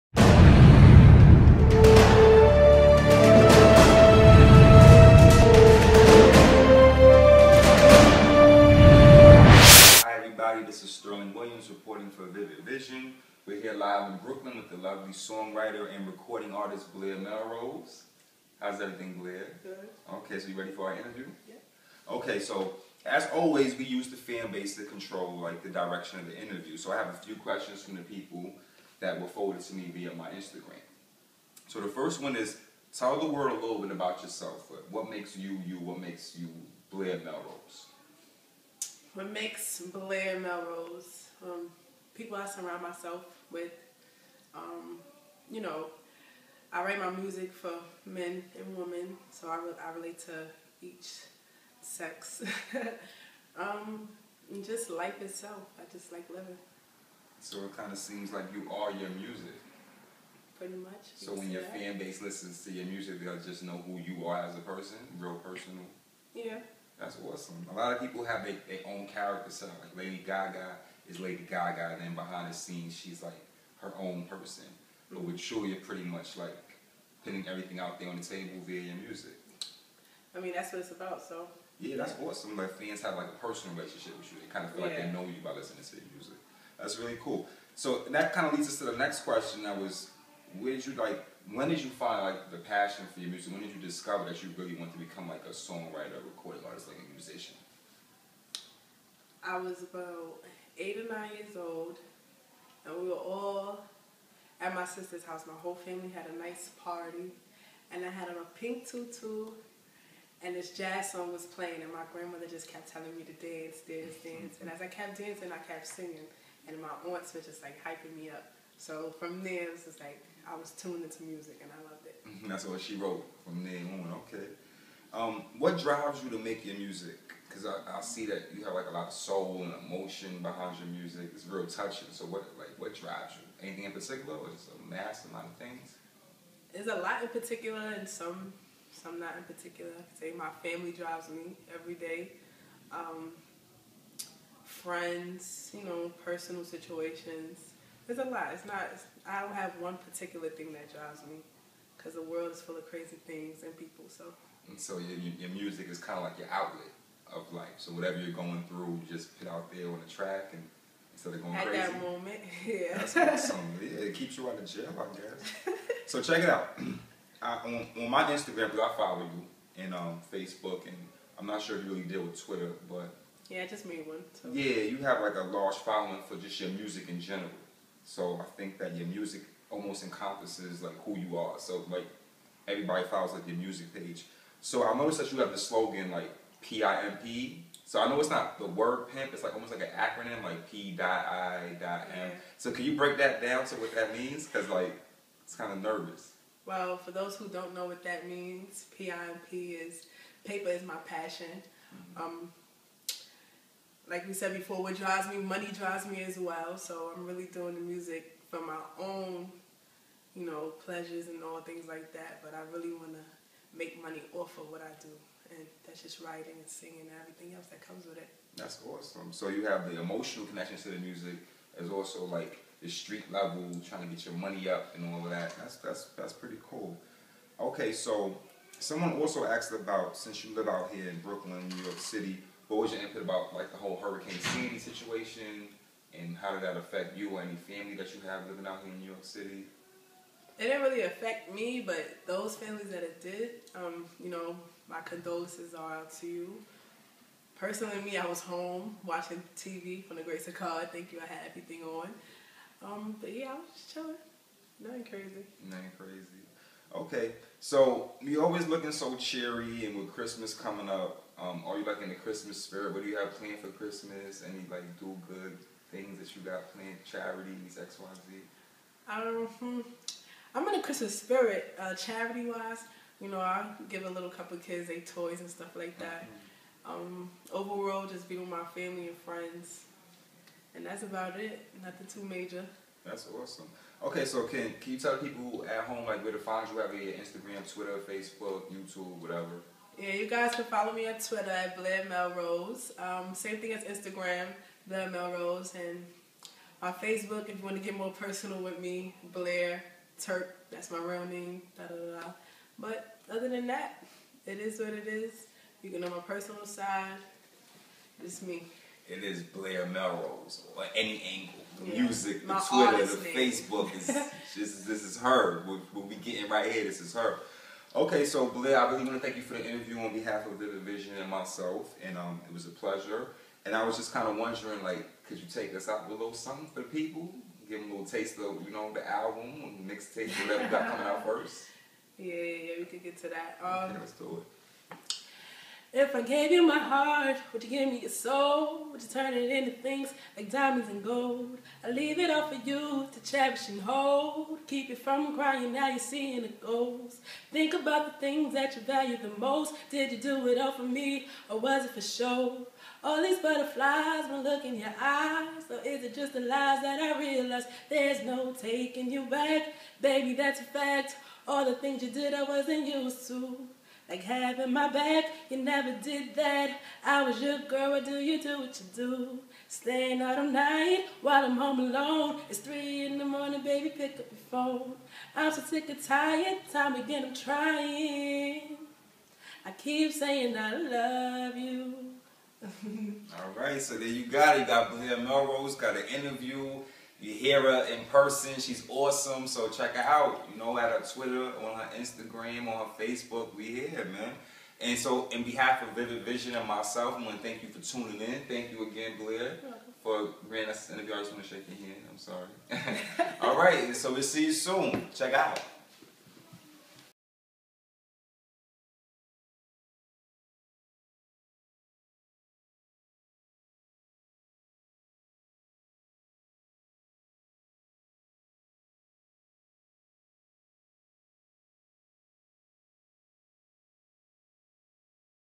Vivid Vision/Buzz on the block
The interview young talented songwriter R&B artist BlairMelrose.
Interview by:Sterling Williams
Camera/Editing by:Marlon"Boro"Patterson
Manage by:Brandi Walker & Marlon"Boro"Patterson